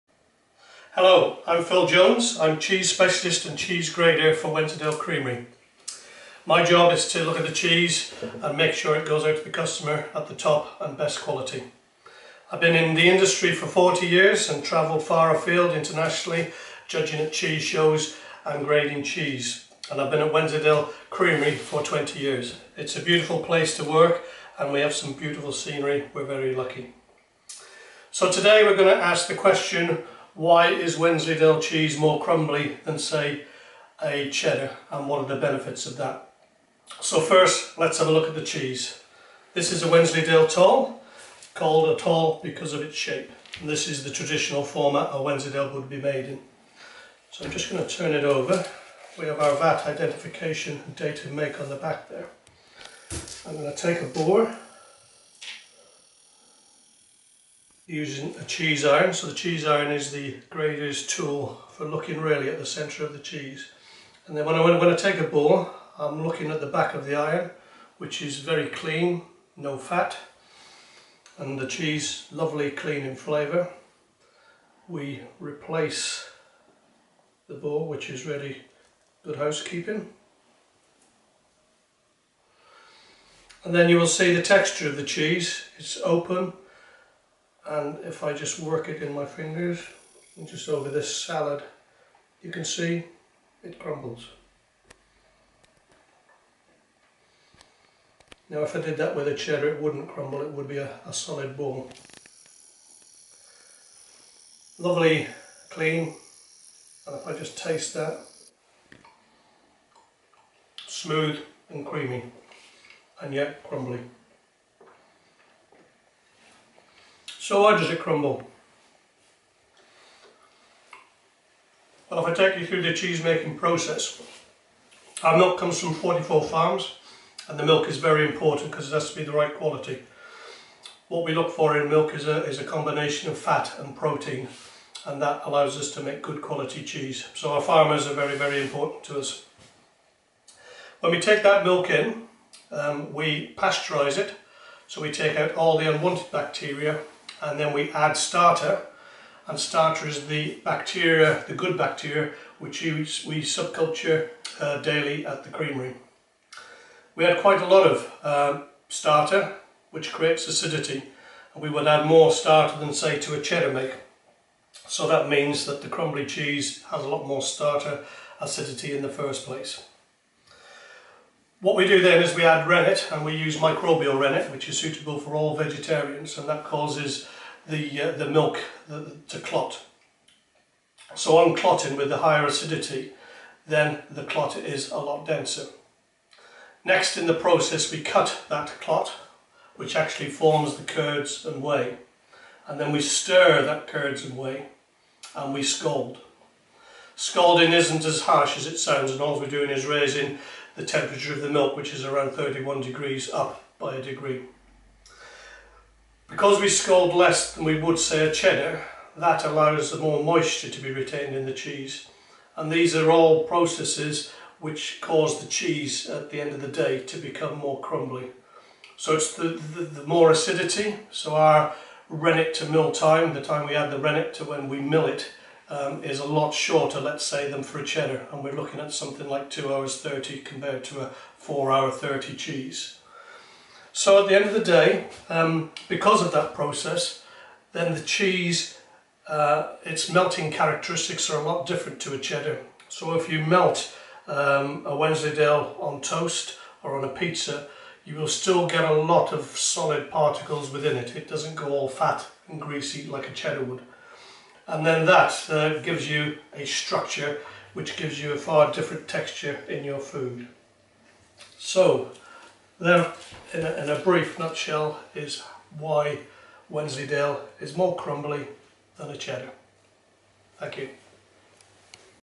Have you ever wondered why Yorkshire Wensleydale crumbles?
Creamy, crumbly and full of flavour…we’ve been handcrafting delicious Yorkshire Wensleydale cheese for generations. The secret to the popularity of this classic cheese? A distinctive crumbly texture, steeped in history and years of knowledge, skill and craftsmanship, passed down through generations of expert cheese-makers that is now widely celebrated.